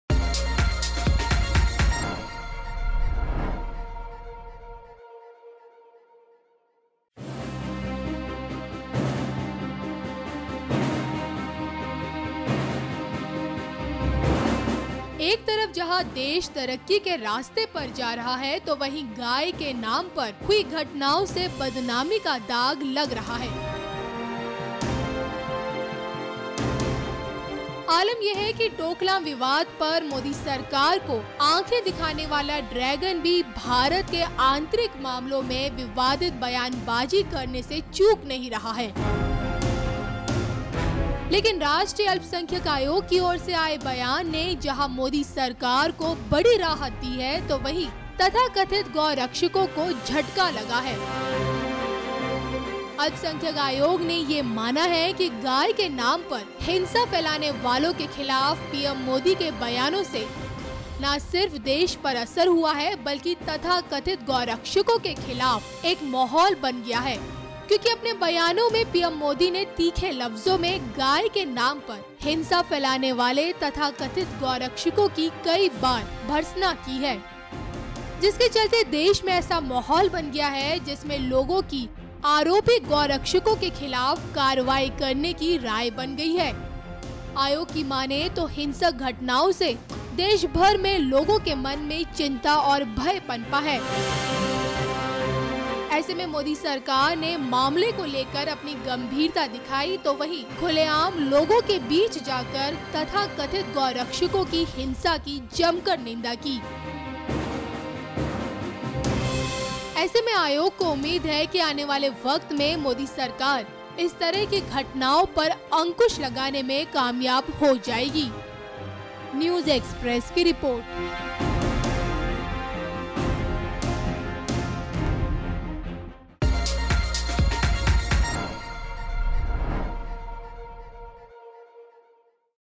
Modi के तीखी बयानों से देशभर में गौरक्षकों के खिलाफ बना माहौल
National minority commission has compliment modi govt on taking steps against Gau Rakshaks . According to national minority commission pm modi condemn violent activities of gau rakshak on the name of cow.
producer - Kavita
video editor - Vineet Singh

राष्ट्रीय अल्पसंख्यक आयोग की ओर से आए बयान ने जहां मोदी सरकार बड़ी राहत दी है तो वहीं तथा कथित गौरक्षों को झटका लगा है।गाय के नाम पर हिंसा फैलाने वालों के खिलाफ पीएम मोदी के बयानों से ना सिर्फ देश पर असर हुआ है बल्कि तथाकथित गौरक्षकों के खिलाफ एक माहौल बन गया है।